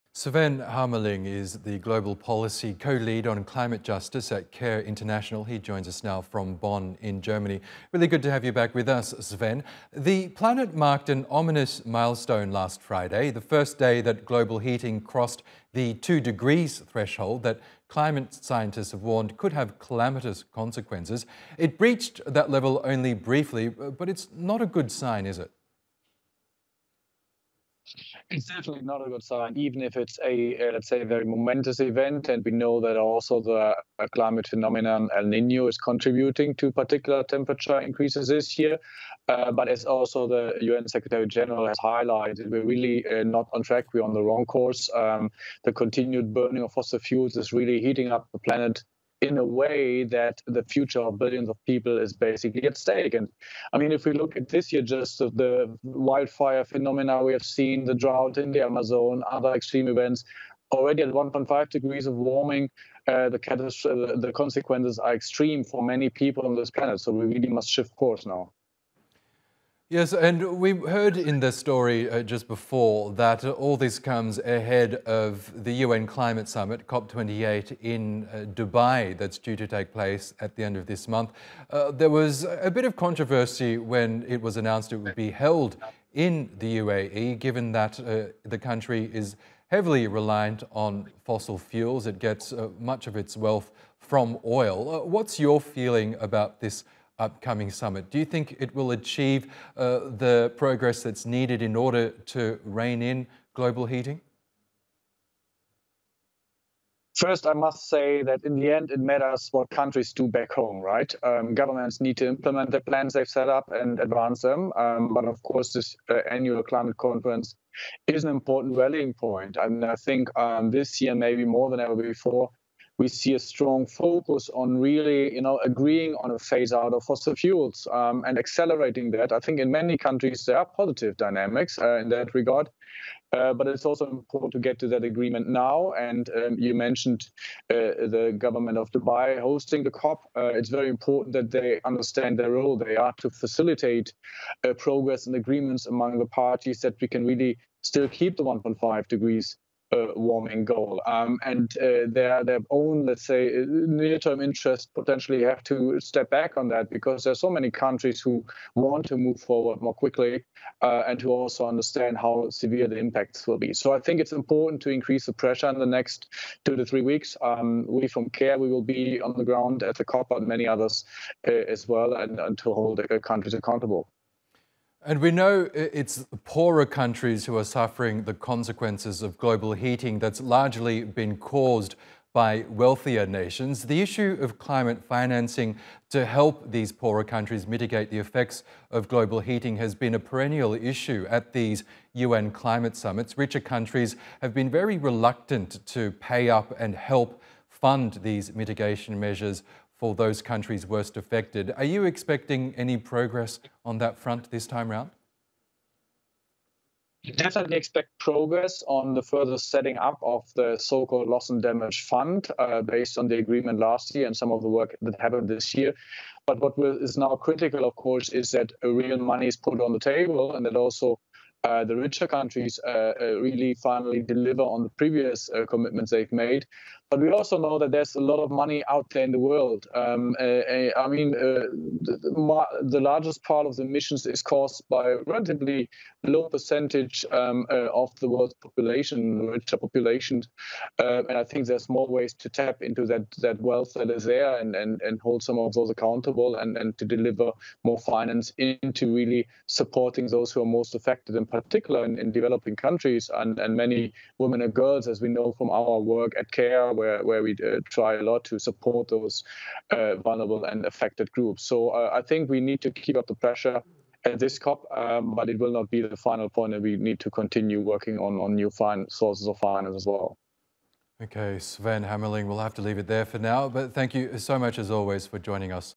UN warns of 3-degree rise in temperature this century
In a new report on Monday, the UN has warned that if the world continues along its current path, it's looking at nearly three degrees of warming this century. Previously, the organisation had warned to keep temperatures from rising no more than one-and- a-half degrees Celsius to avoid the worse effects of the climate crisis. Sven Harmeling from CARE International weighs in